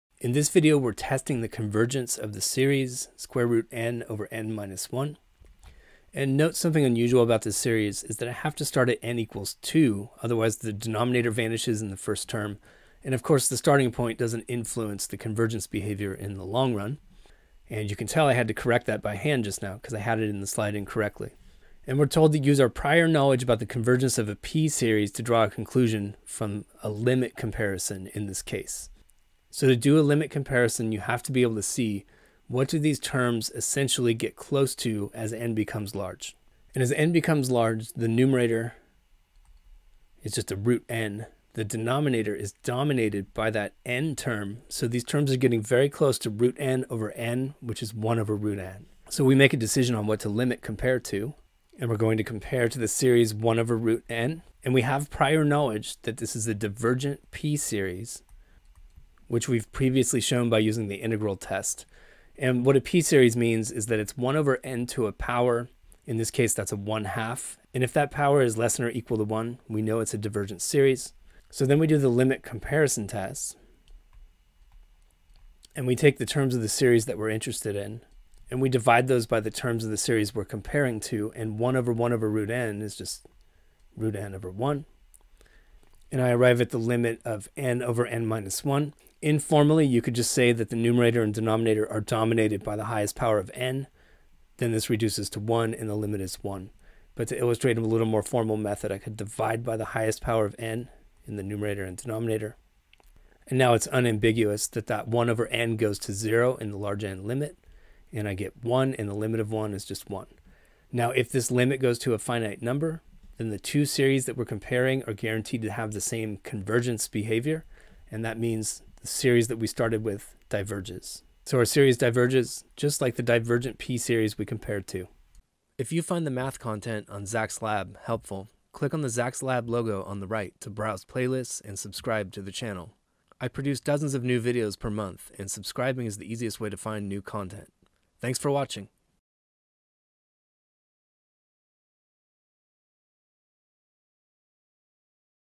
Limit comparison test sqrt(n)/(n-1), limit comparison to divergent p-series 1/sqrt(n).
In this problem, we check the convergence of sqrt(n)/(n-1) using a limit comparison.  We begin by thinking about the large n limit of the terms, and we can see that they become approximately equal to 1/sqrt(n).  This gives us the idea to do a limit comparison to 1/sqrt(n), which is a divergent p-series, since the exponent of n in the denominator is less than 1.

We perform the limit comparison, and we arrive at a finite limit of the ratio of terms.  This means both series share the same convergence behavior, and our series diverges as well.